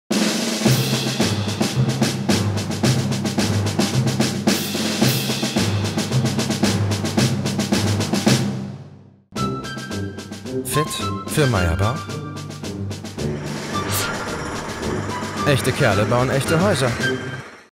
Mayr Bau: Fun Facts vom Bau
„So flink und zuverlässig wie die Ameisen“. Das sind die Mitarbeiter bei Mayr Bau. Man kann sich getrost diese kleinen Baumeister als Vorbild nehmen, den auch wir sind überall dort zu Hause, wo gebaut wird, Schnelligkeit, Verlässlichkeit und TOP QUALITÄT gefragt ist.
MAYR BAU -  ECHTE KERLE BAUEN ECHTE HÄUSER